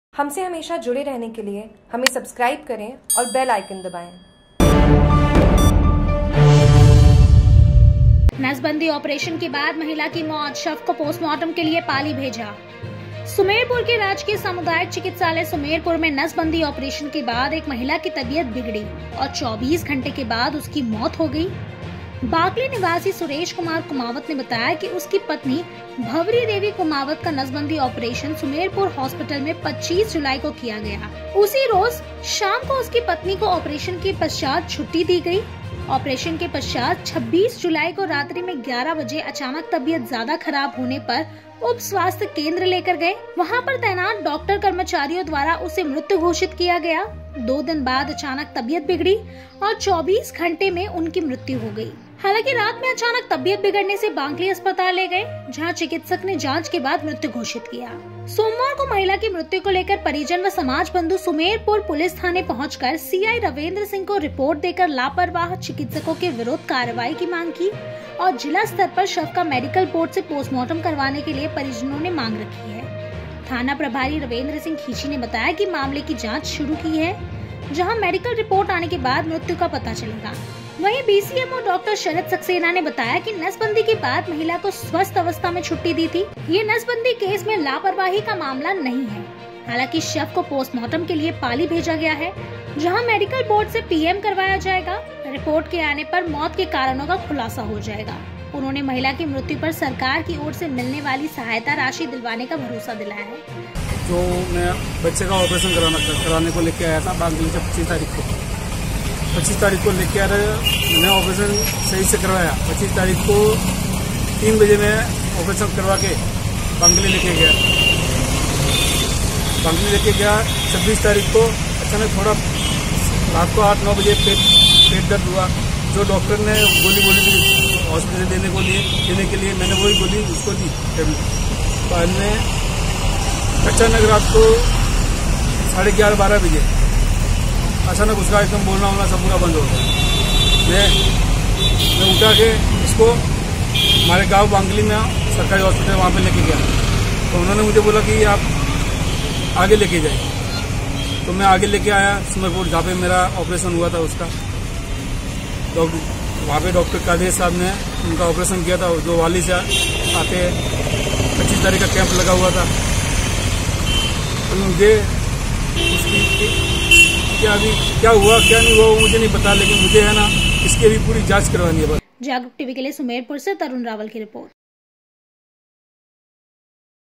Jagruk TV सुमेरपुर : नसबंदी ओपरेशन के बाद महिला की मौत, शव को पोस्टमार्टम के लिए पाली भेजा
सुमेरपुर के राजकीय सामुदायिक चिकित्सालय सुमेरपुर मे नसबंदी ओपरेशन के बाद एक महिला की तबियत बिगडी और 24 घंटे के बाद उसकी मौत हो गई।

बाकली निवासी सुरेश कुमार कुमावत ने  बताया कि उसकी पत्नी भंवरीदेवी कुमावत का नसबंदी ओपरेशन सुमेरपुर होस्पीटल मे 25 जुलाई को किया गया। उसी रोज शाम को उसकी पत्नी को ऑपरेशन के पश्चात छुट्टी दी गई।

 ऑपरेशन के पश्चात 26 जुलाई को रात्रि में 11 बजे अचानक तबीयत ज्यादा  खराब होने पर उप स्वास्थ्य केंद्र लेकर गए। वहां पर तैनात डॉक्टर कर्मचारियों द्वारा उसे मृत घोषित किया गया। दो दिन बाद अचानक तबियत बिगडी और 24 घंटे में उनकी मृत्यु हो गई। हालांकि रात में अचानक तबीयत बिगड़ने से बाकली  अस्पताल ले गए, जहा चिकित्सक ने जांच के बाद मृत घोषित किया।

 सोमवार को महिला की मृत्यु को लेकर परिजन व समाजबंधु  सुमेरपुर पुलिस थाने पहुच कर सीआई रविंद्र सिंह को रिपोर्ट देकर लापरवाह चिकित्सकों के विरूद्ध कारवाही की माग की और जिला स्तर पर शव का मेडिकल बोर्ड से पोस्टमार्टम करवाने के लिए परिजनो ने मांग रखी है। थाना प्रभारी रविन्द्रसिह खीची ने बताया कि मामले की जाच शुरू की है,जहां मेडिकल रिपोर्ट आने के बाद मृत्यु का पता चलेगा। वही बीसीएमओ डॉक्टर शरद सक्सेना ने बताया कि नसबंदी के बाद महिला को स्वस्थ अवस्था मे छुट्टी दी थी। यह  नसबंदी केस मे लापरवाही का मामला नही है।

हालांकि शव को पोस्टमार्टम के लिए पाली भेजा गया है, जहां मेडिकल बोर्ड से पीएम करवाया जाएगा रिपोर्ट के आने पर मौत के कारणों का खुलासा हो जाएगा। उन्होंने महिला की मृत्यु पर सरकार की ओर से मिलने वाली सहायता राशि दिलवाने का भरोसा दिलाया है।

सुमेरपुर से तरुण रावल की रिपोर्ट

Jagruk TV